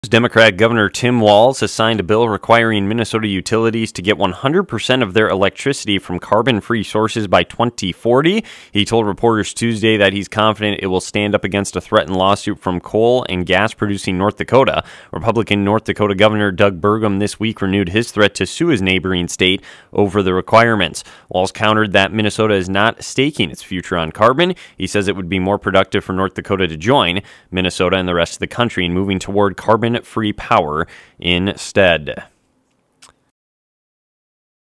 Possible Lawsuit After MN Passes Carbon Free Legislation
ST. PAUL, MN (KROXAM.com) A possible lawsuit by North Dakota against Minnesota is brewing, after Governor Walz signed carbon free energy legislation.